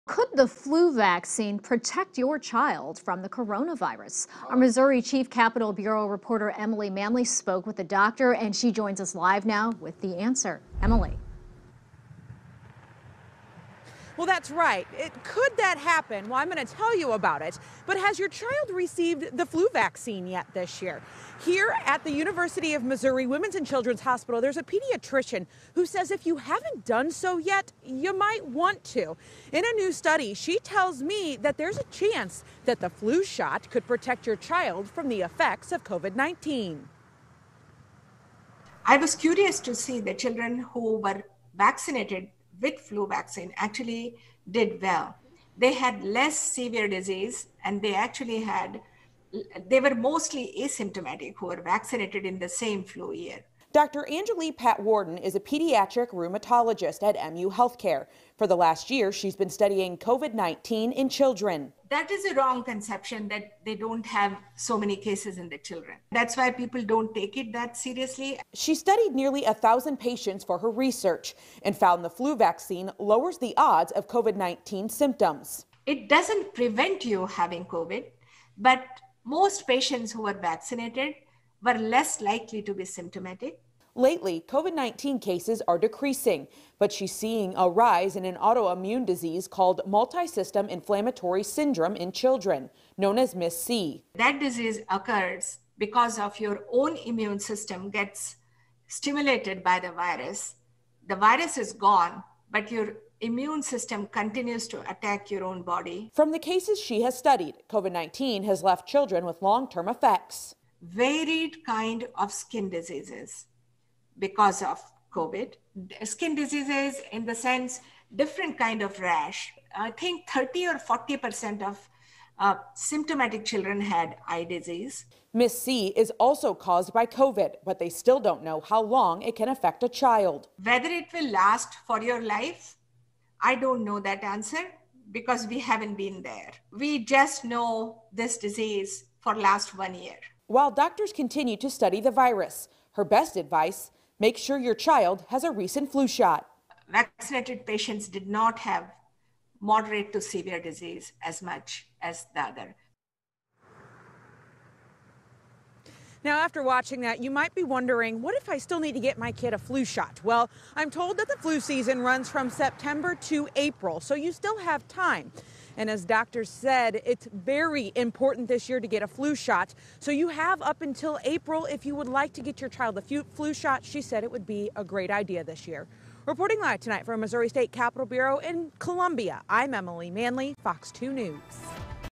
Doctor says flu vaccine could protect kids from the effects of COVID-19
A pediatrician wants parents to know the flu vaccine could protect your child from life-long complications from coronavirus.